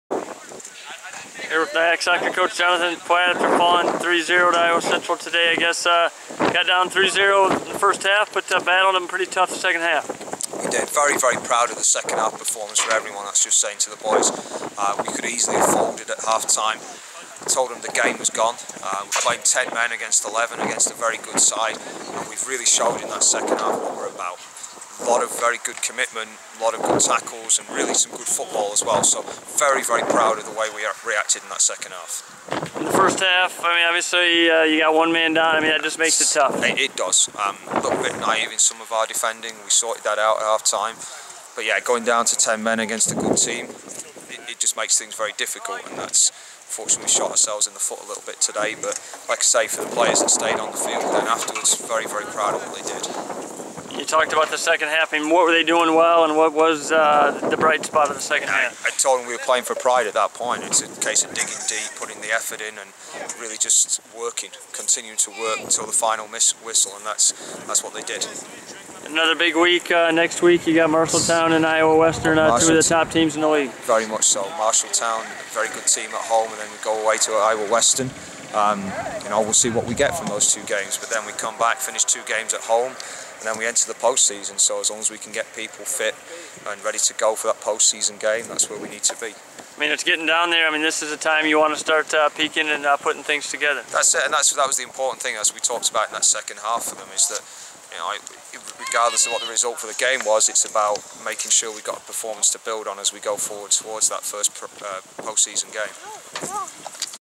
NIACC's Jonathan Platt
NIACC soccer coach Jonathan Platt talks after his team dropped a 3-0 decision to Iowa Central on Saturday in Mason City.